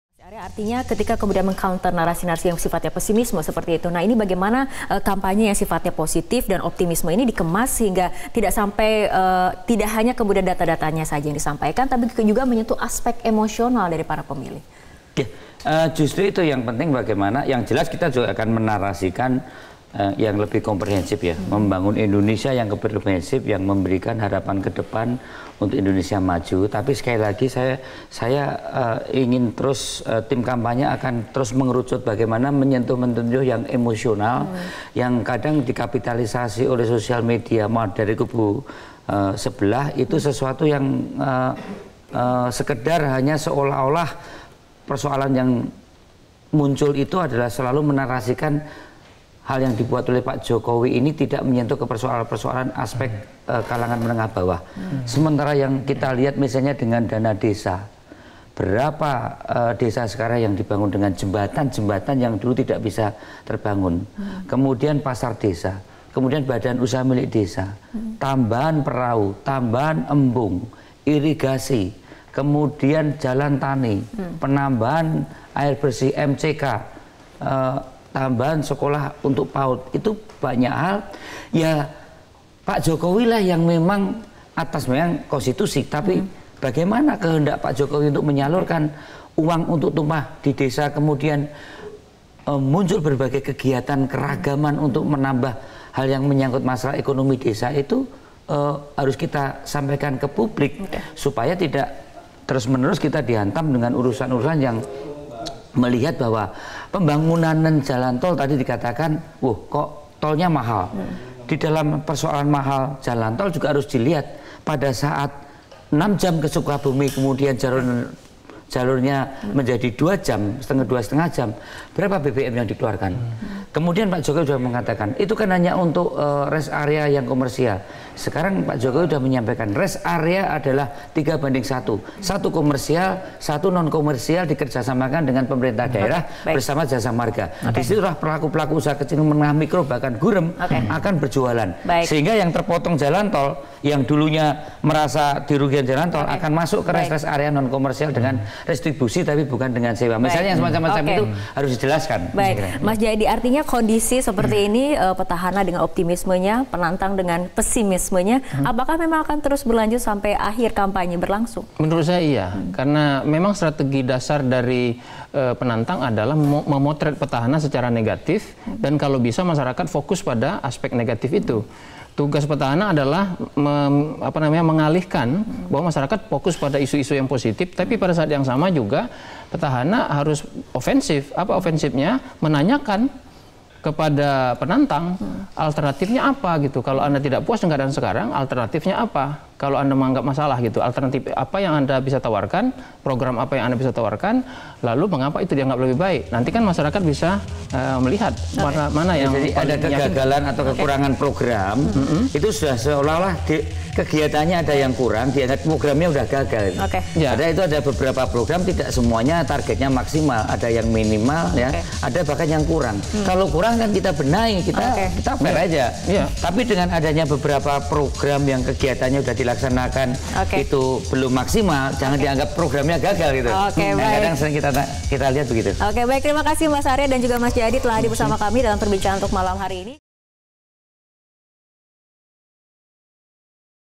SMRC: Saatnya Petahana Ofensif ke Penantang yang Selalu Memotret Negatif Petahana
Dua kubu peserta Pilpres 2019 diperkirakan akan menggunakan strategi yang sama hingga hari pencoblosan nanti. Kubu Jokowi-Ma'ruf disarankan untuk menantang kubu Prabowo-Sandi untuk menunjukkan program kerja jika terpilih.